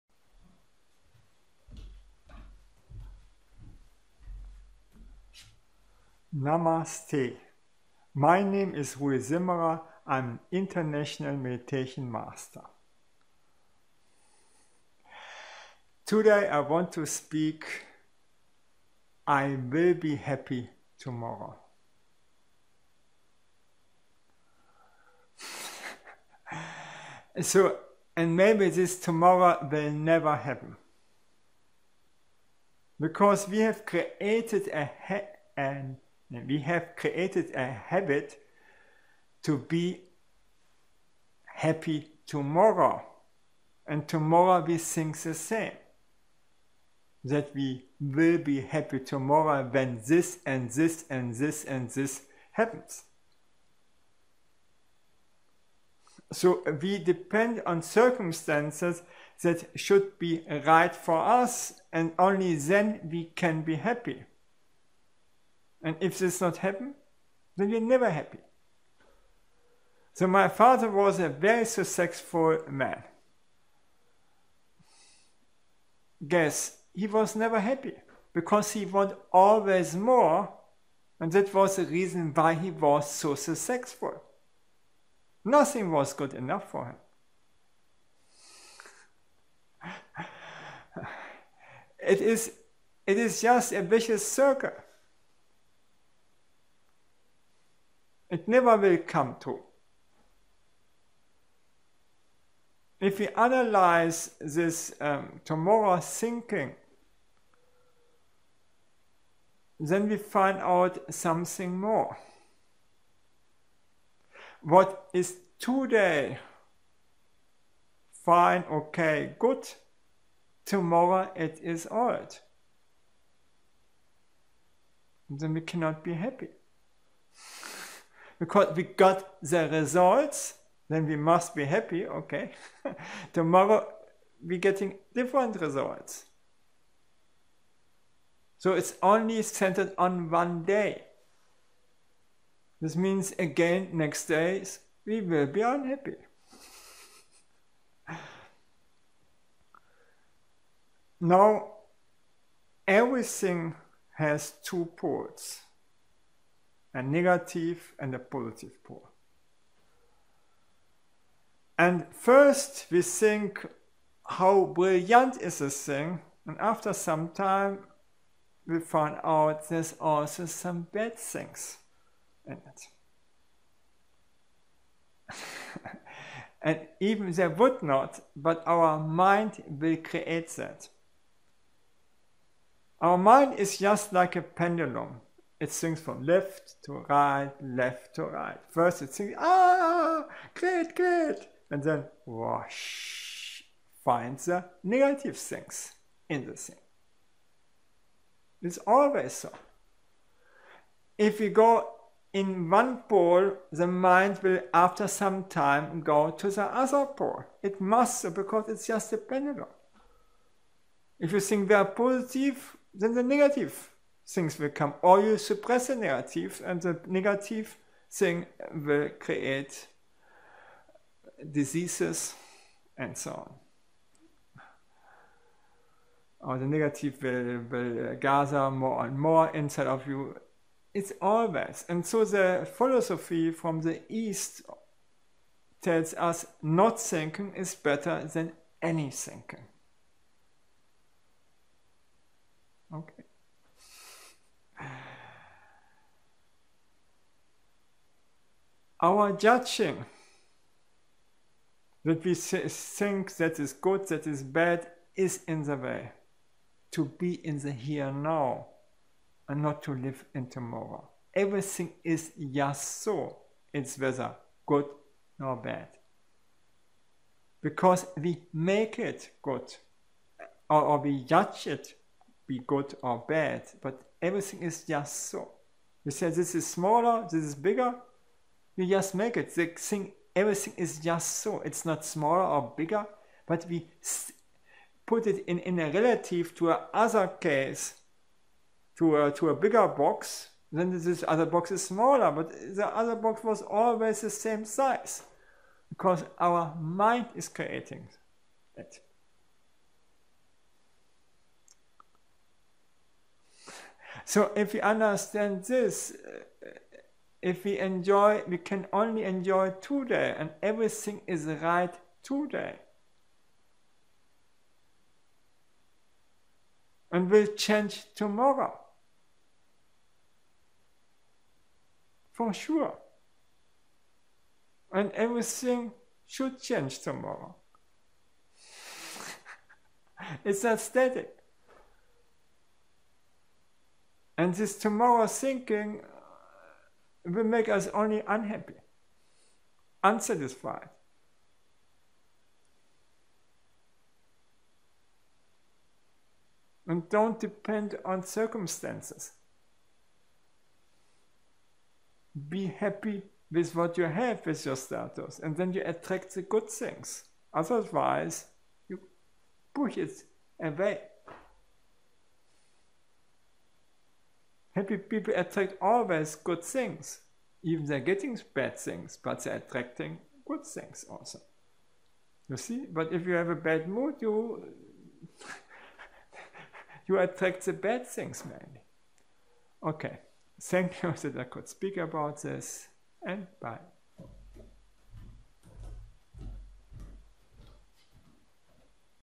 I’ll be happy tomorrow
… If… And that maybe never happens.
When we get even everything fulfilled, we will still be unhappy. Because we have created this habit to be happy tomorrow.

My father who was a very successful guy was never happy. Because he always wanted more. And that was the reason he was so successful.

All the right things happen today and never tomorrow.

The Tomorrow thinking is a vicious circle. Because the tomorrow will be the same of postponing to be happy. And even you will be happy tomorrow, the next day you will again shift the happiness on tomorrow.

Now we want to analyze this tomorrow thinking:

What we gain tomorrow is in 2 days old again…. And then we are unhappy again.
When we get something new, then we think first how brilliant that thing is. Everything has 2 poles…. A beneficial pole and a negative pole. Our mind works like a pendulum. From positive it will swing to negative. Even we admire something after some time our mind will look for mistakes. And that is the reason why in the East philosophy/religion to stop thinking better is than to think at all.
We think we can control tomorrow. Normally we have maybe 5% under our control. Every moment we can die… And that will happen!

Everything is just so, it is neither good or bad. Our thinking makes a thing positive or negative. Still, every thing is just so.

How is it if we analyze every thing that we want for the positive and negative.  And this I do since over 30 years.

I found out that mostly what I desire is not so attractive as I thought before. And guess what happens?

I can be easier happy with what I have and live so more in the here and now.

The more things change so more they stay the same.
This means we remain the same, regardless how many things are changing.

How is it to be happy with the things, status you have already? The tomorrow will never be better! Don’ t block your happiness through your Tomorrow thinking.